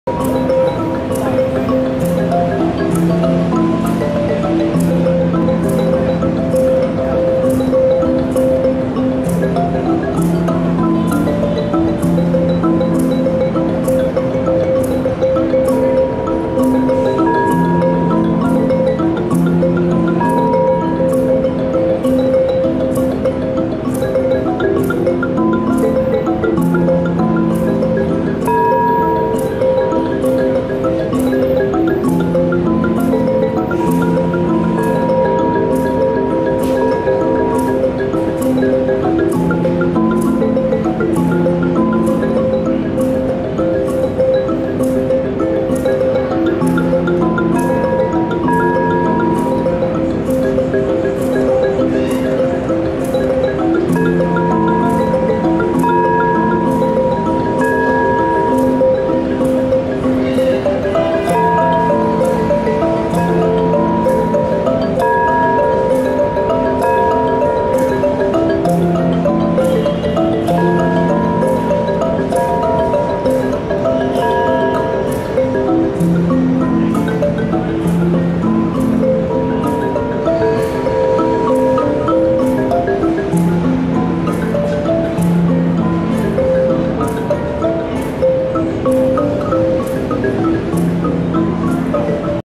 Sonic Light Mbira Meditation (Union Square NYC)
Sending out the light for health freedom
with the Sonic Light Prayer Meditation
last Friday (Nov 4th) broadcasting sonic light from Union Square.
Sending the light to the health freedom candidates and into this upcoming election.
The wind and the trees join the prayer for health freedom.
Sending out healing light and healing freedom to everyone!
Saco Yasuma on mbira
Kevin Nathaniel on mbira
Alma on Tibetan bowl
Gregory Kage on rattles
Ghost on guitar
and light continues to flow throughout the universe
One love...